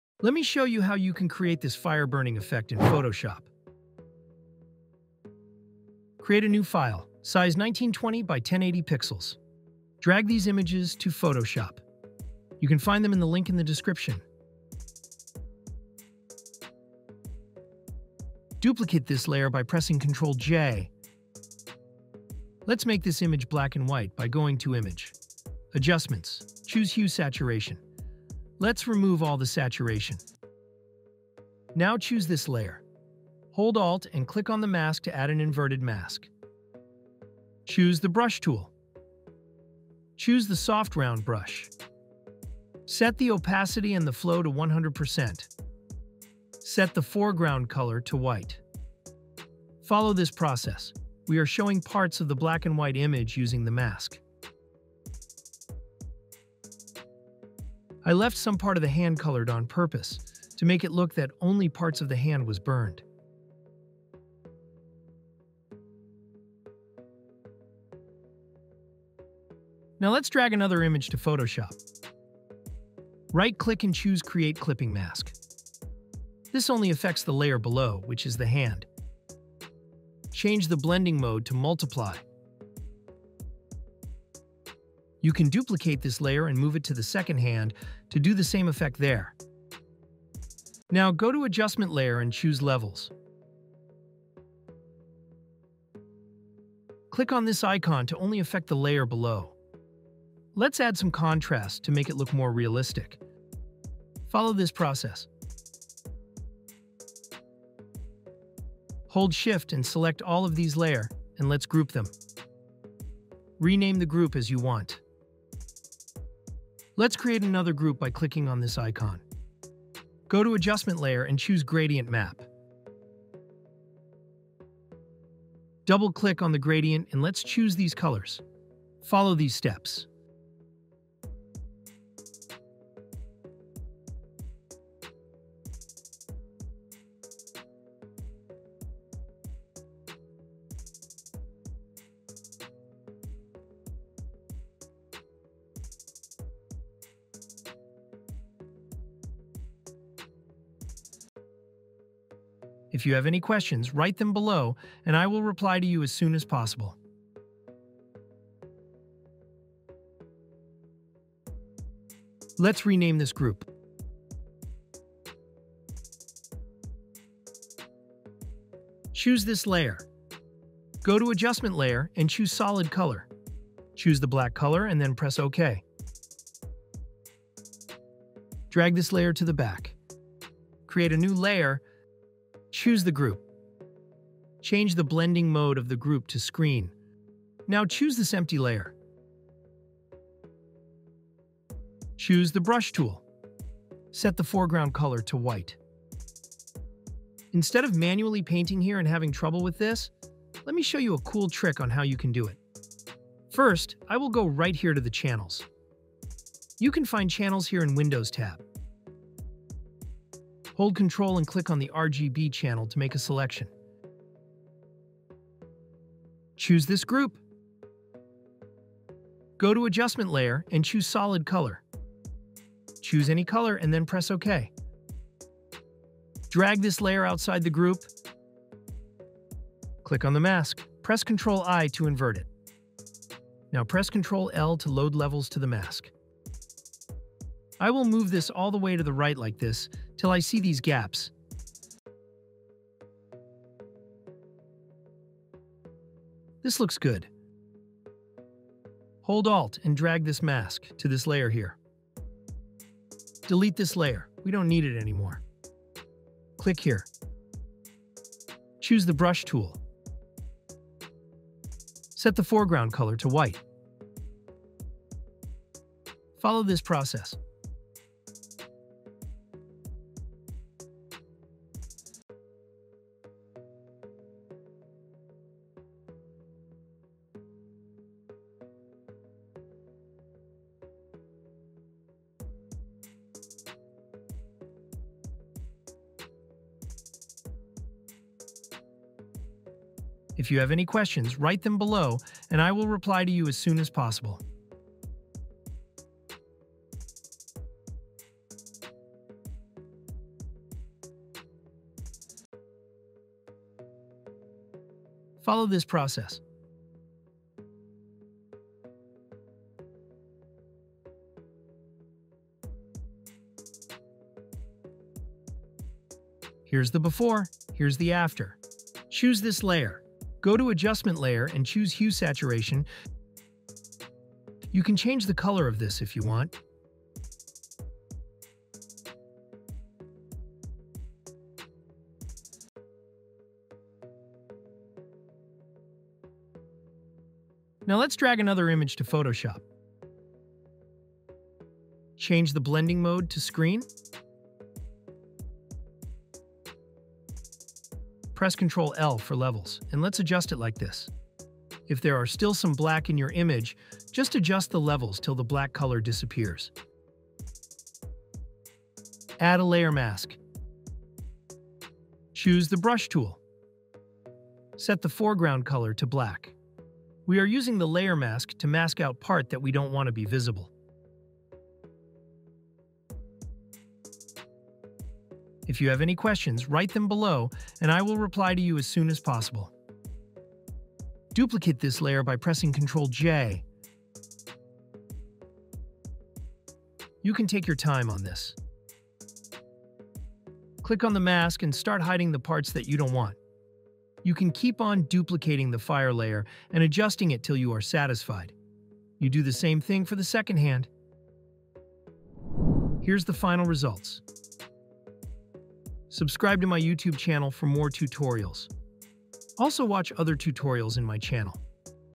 Create A Stunning Fire Effect In Photoshop
In this tutorial, I will show you how to create Fire Burning Effect in Photoshop. i've learned this from @NemanjaSekulic